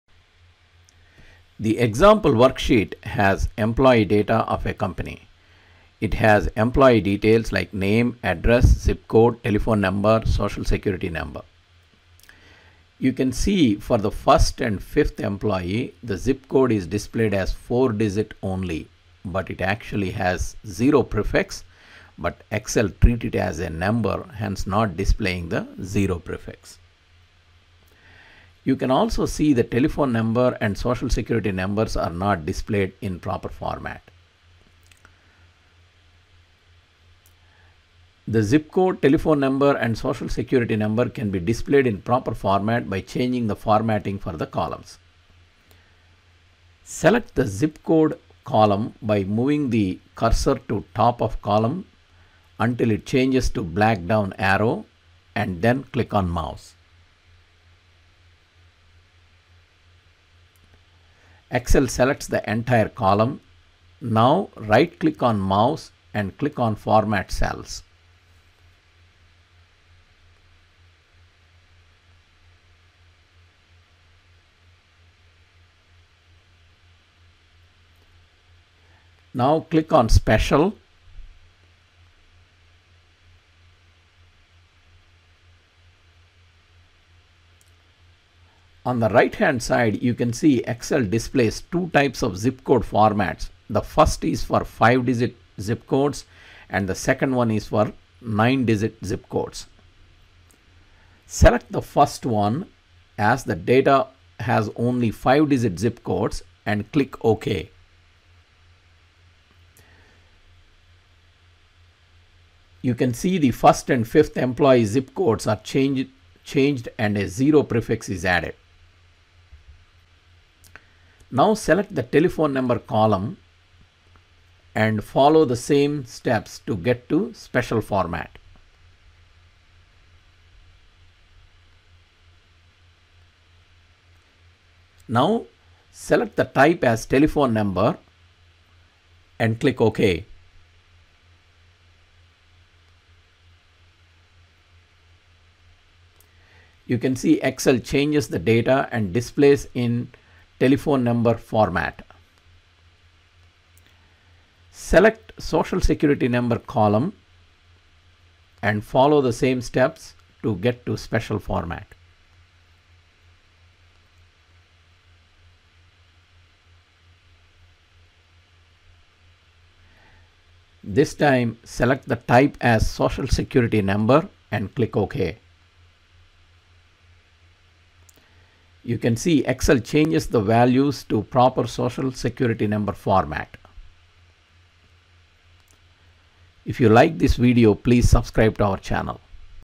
How to display Zip Codes, Telephone Numbers and SSN in proper format in Excel - Office 365
How to display Zip Codes, Telephone Numbers and SSN in proper format in Excel - Office 365. This video explains how to change formatting for columns which have zip code, telephone number and social security number data so that data is displayed in proper format.